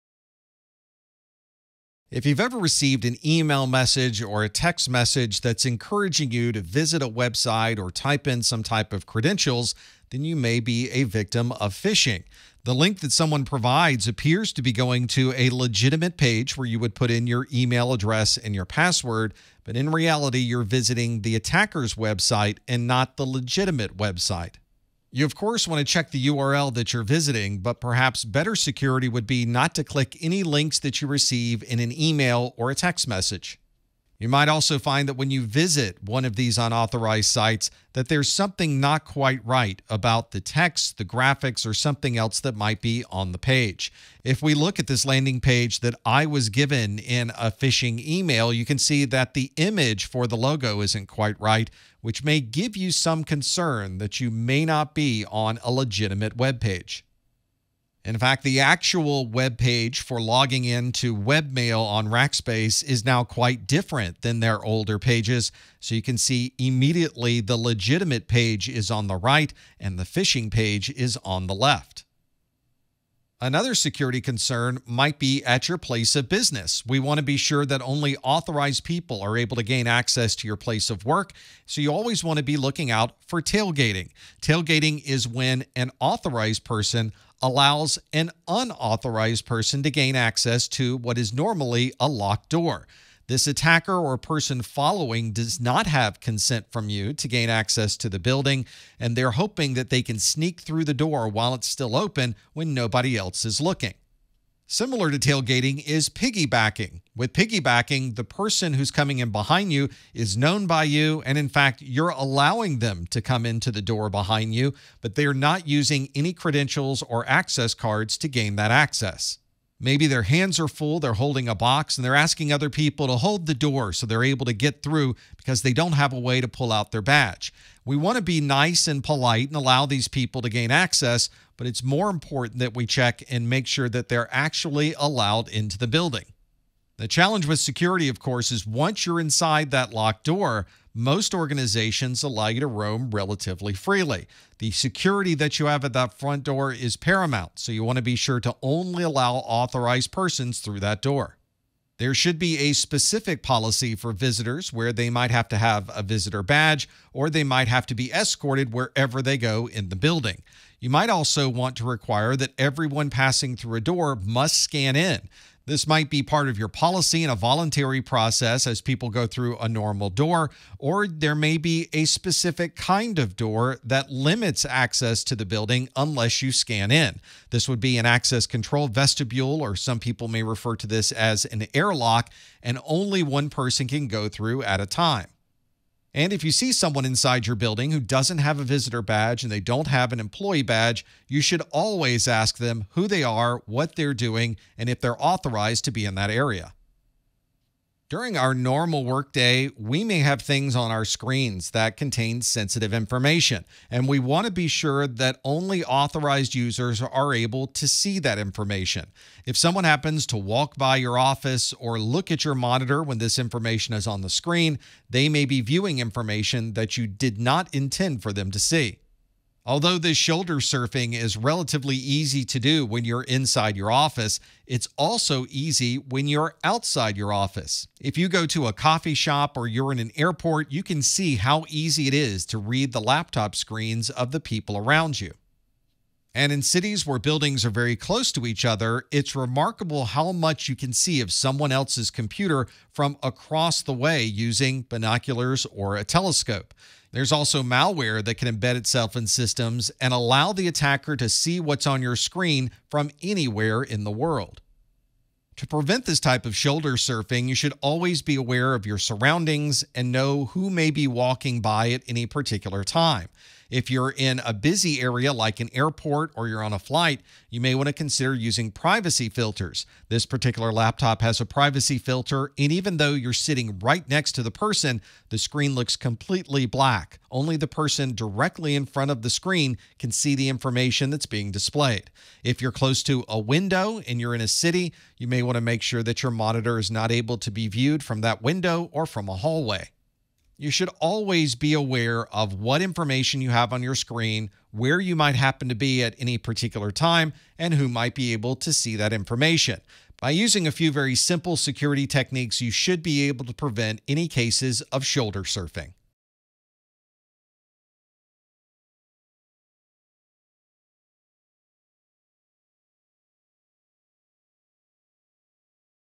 Social Engineering - N10-008 CompTIA Network+ : 4.2
Attacks often use social engineering to gain access to a physical building or confidential data. In this video, you’ll learn about phishing, tailgating, piggybacking, and shoulder surfing.